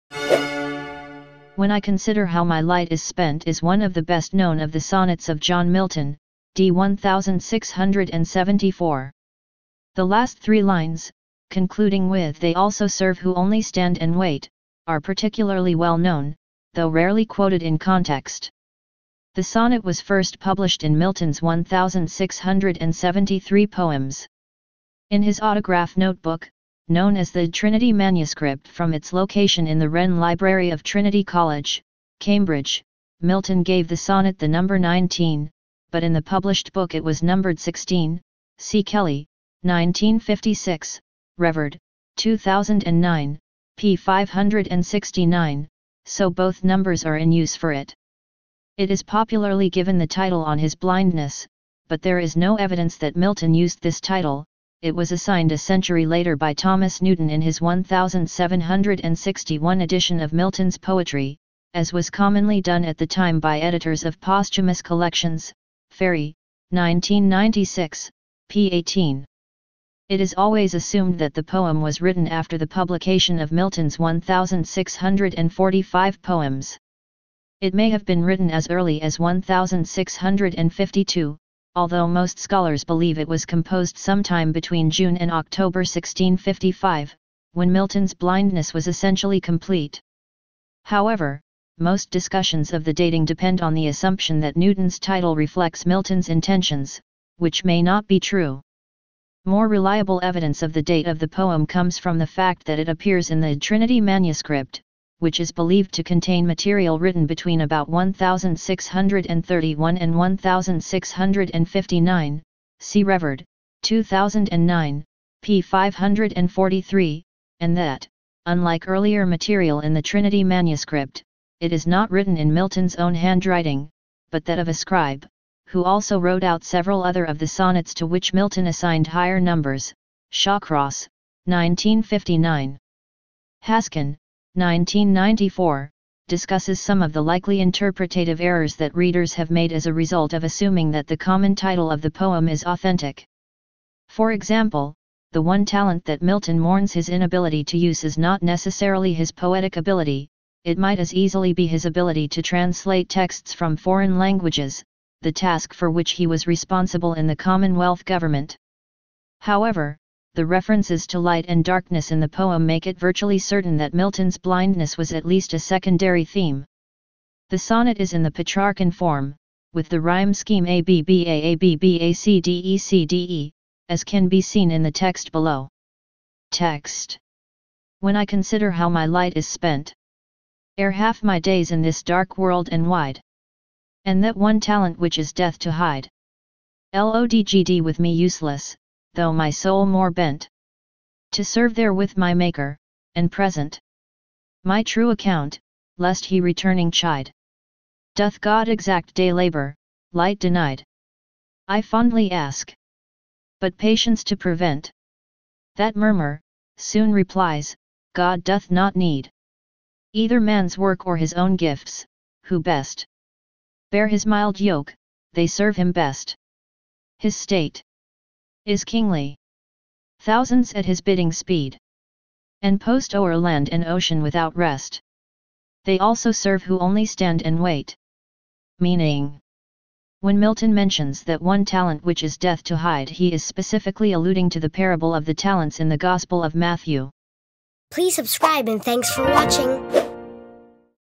When I Consider How My Light is Spent (Your EYEBALLS) 👁️👁️💉😳💊🔊💯✅
This video is about "When I Consider How My Light is Spent". This video series is something special. We're fully delving into all things everything and all things about the HUMAN EYE!